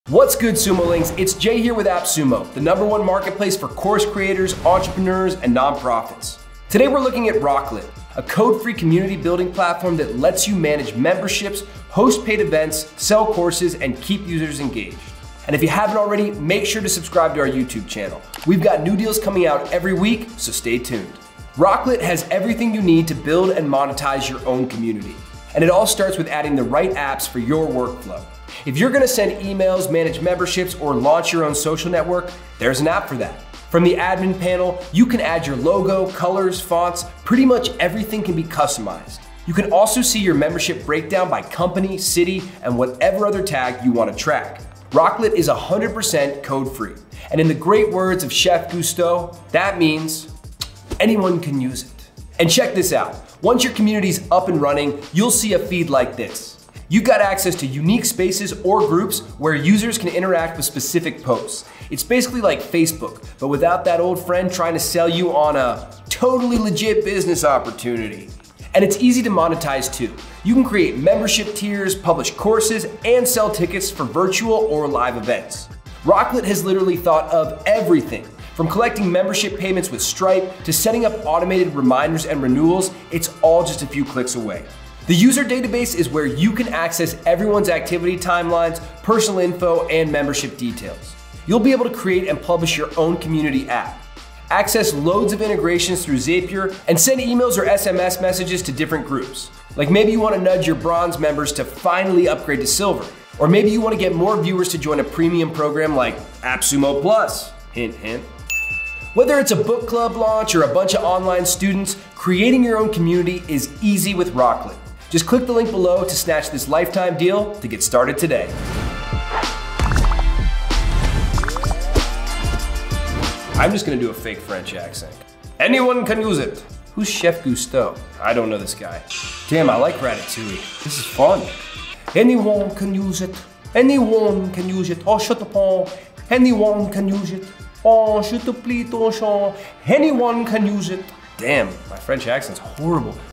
Build Your Own Paid Community and Mobile App with Raklet (No Coding!)
Raklet is a code-free community-building platform that lets you manage memberships, host paid events, sell courses, and keep users engaged.

Social media platforms aren’t always the best place to build your own online community. (“Who knew our biggest competition would be viral cat videos?”)

While those platforms are great for building an audience, they’re not built to help you manage user data or sell memberships.

That’s why you need a no-code platform that lets you build your own social network with the exact features and membership plans you want.

Introducing Raklet.